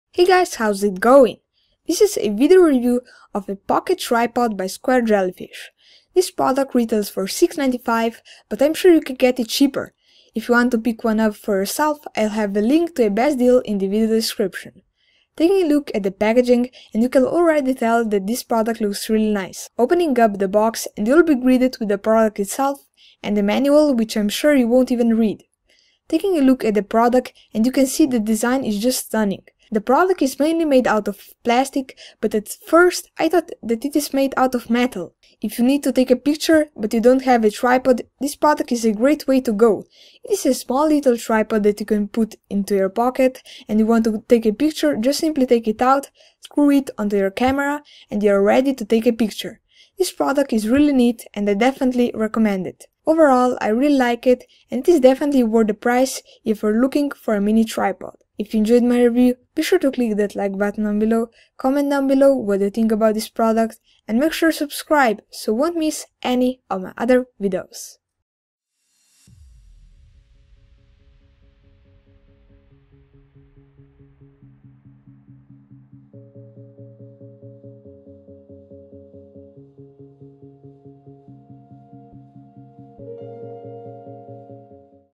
Square Jellyfish Pocket Tripod Full Review
This is my video review of a Pocket Tripod by Square JellyFish. It retails for $6.95, but I'm sure you can get it cheaper. As the name says this product is pretty much a mini tripod that you can put into a pocket. Anyway here's my full review.

Pocket Tripod (US)

Best Deals in Tech (US)
Best Deals in Tech (UK)